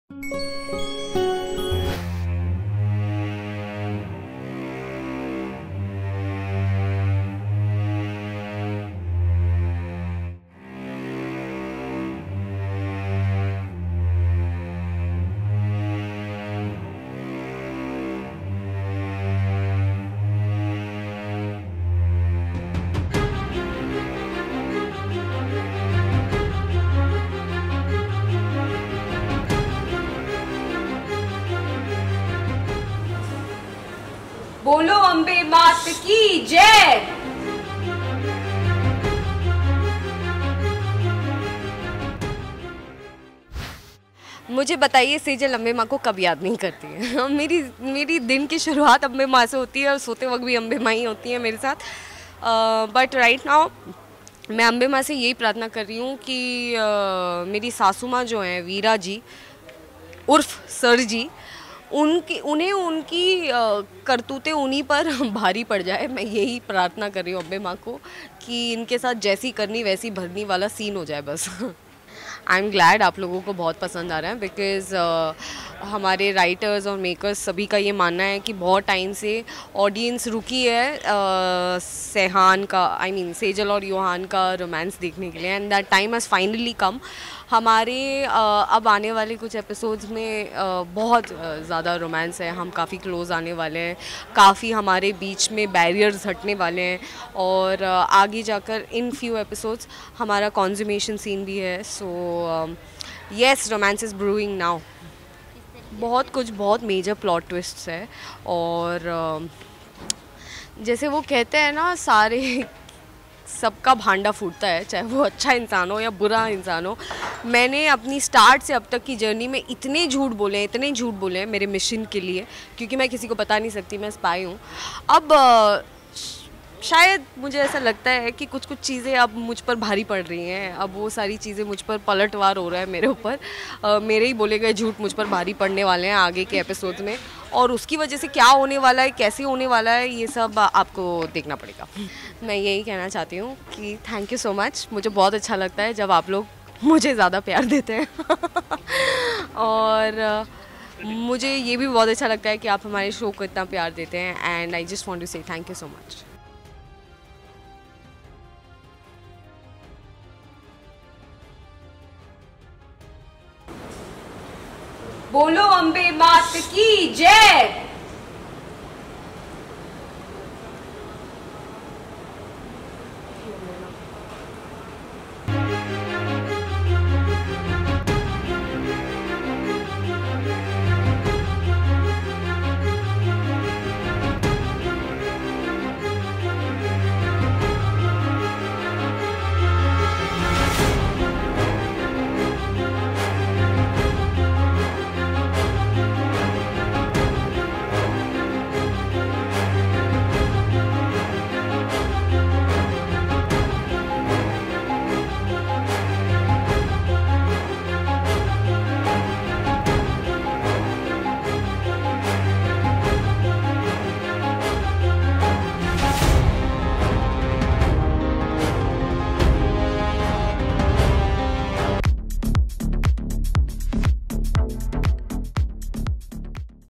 Spy Bahu On Set: Veera aka Sirji Ka Plan Hua Fail, Sejal Ka Juth Padega Sejal Par Bhari
Colors Tv Spy Bahu On Location: Veera aka Sirji Ka Plan Hua Fail, Sejal Ka Juth Padega Sejal Par Bhari